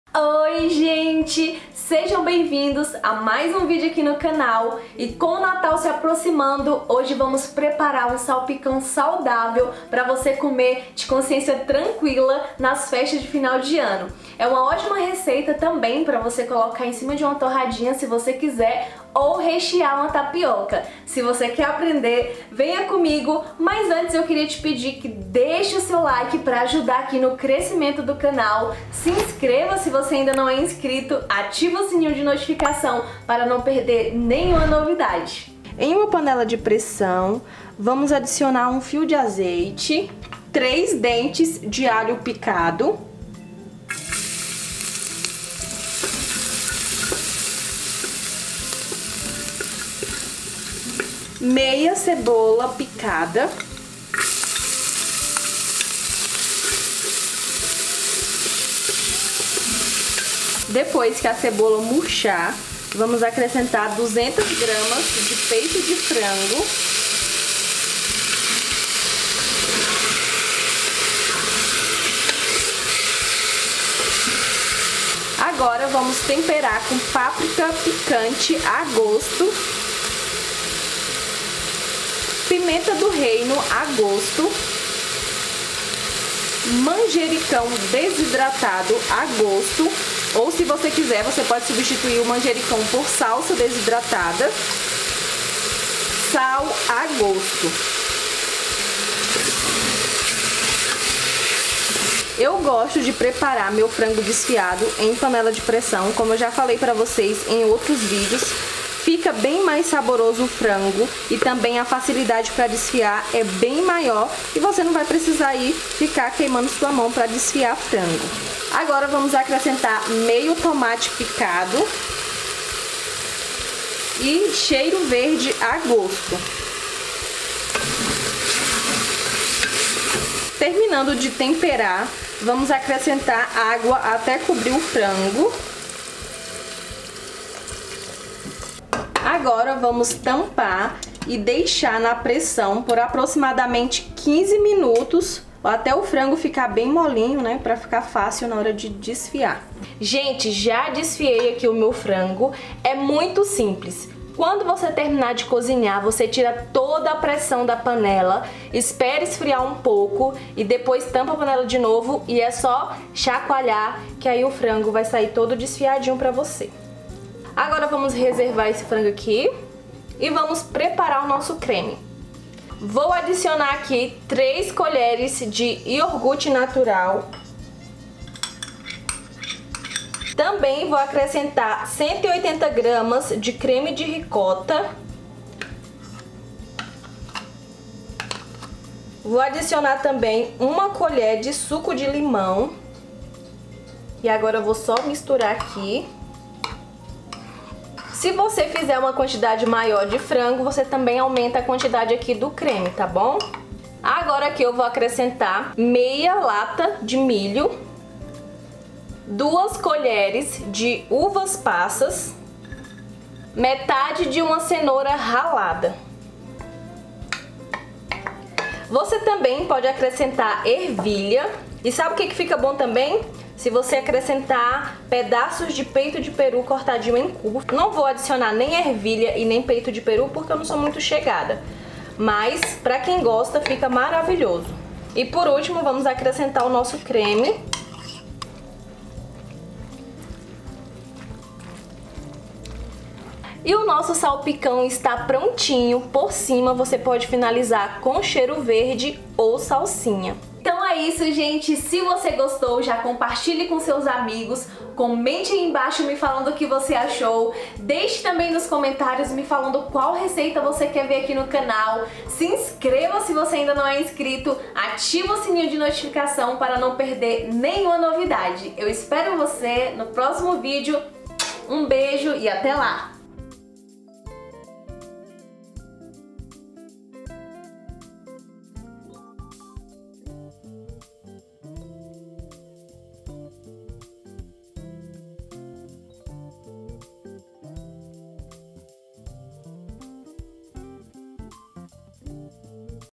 SALPICÃO DE FRANGO SEM MAIONESE FÁCIL E SAUDÁVEL/ Especial de Natal
Você só vai querer comer salpicão assim👇🏽
Salpicão de Frango Fácil Sem Maionese🗣️

Com as Festas de Final de Ano se aproximando, nada melhor que uma receita de salpicão de frango saudável, sem nada de maionese e fácil de fazer. GOSTOU DA RECEITA?

》VEJA OUTRAS RECEITAS DE NATAL AQUI DO CANAL

2OOg- Frango
3-Dentes de alho
1/2- Cebola
1/2- Tomate
1/2- lata de milho
1/2- Cenoura ralada
2c- Uvas-passas
Temperos a gosto
Cheiro verde
Páprica apimentada
Pimenta-do-reino
Manjericão ou Salsa desidratada
Sal
Creme
3c- Iogurte natural
180g- Creme de Ricota
1c- Suco de limão

*ATENÇÃO: Para o passo a passo da receita assiste o vídeo até o final*

➡️ ME SIGA NAS REDES SÓCIAS ⬅️